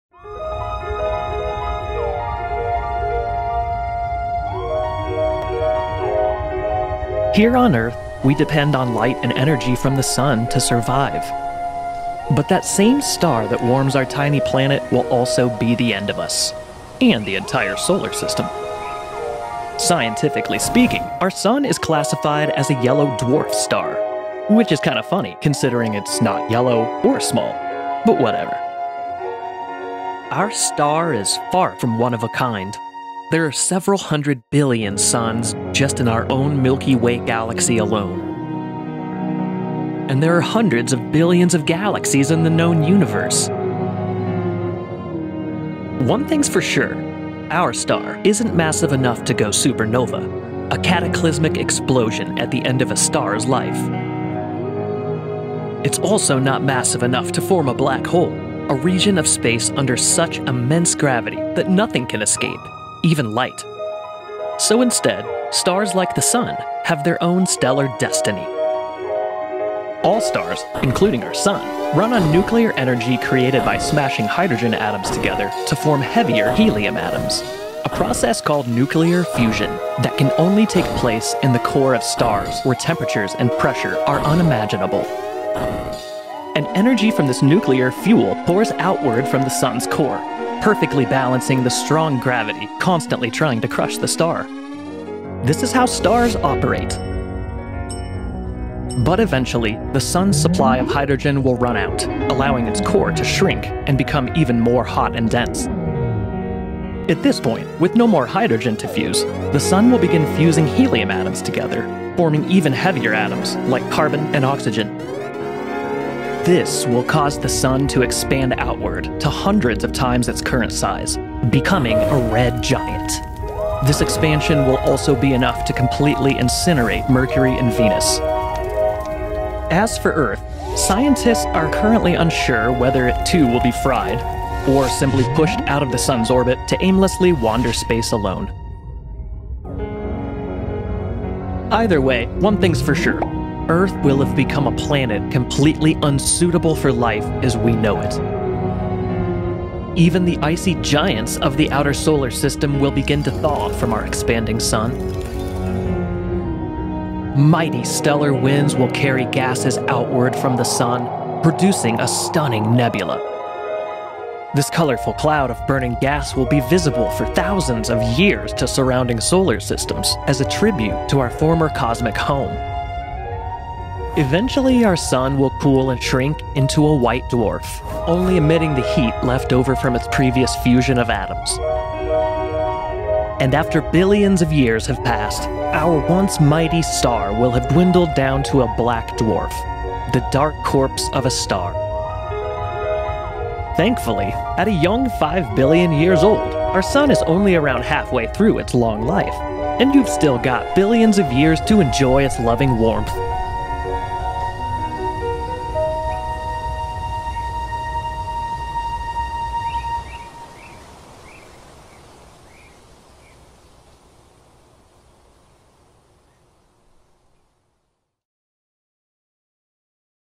Ep 14 How Will Our Solar System End?
Just like we are very curious to understand how the solar system began, we also want to know how it will all end! In this final episode we explore how our entire solar system will eventually end and fade away into the cosmos. Our final mission!